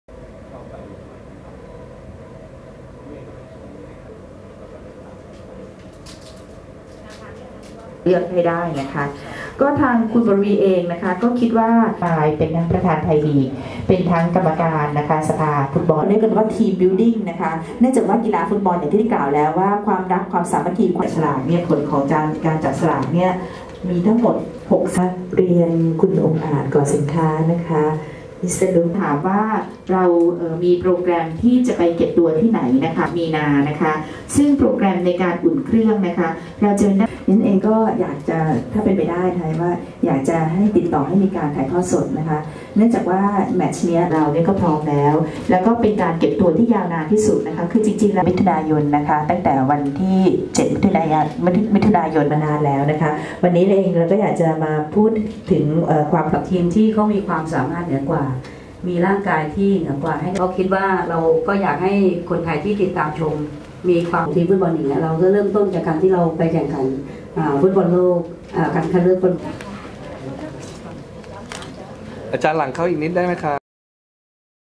นวลพรรณ ผู้จัดการทีมฟุตบอลหญิงแถลงข่าวความเคลื่อนไหวและแผนเตรียมทีม
นวลพรรณ ล่ำซำ กรรมการผู้จัดการและประธานเจ้าหน้าที่บริหาร บ.เมืองไทย ประกันภัย จำกัด(มหาชน) และผู้จัดการทีมฟุตบอลหญิงทีมชาติไทย แถลงข่าวต่อสื่อมวลชนถึงความเคลื่อนไหวและแผนเตรียมทีมฟุตบอลหญิงทีมชาติไทย ก่อนเข้าร่วมการแข่งขันฟุตบอลหญิงชิงแชมป์โลก 2015 ที่ประเทศแคนาดา ในเดือน มิ.ย. 2558Photographer วันชัย ไกรศรขจิต (Wanchai Kraisornkhajit)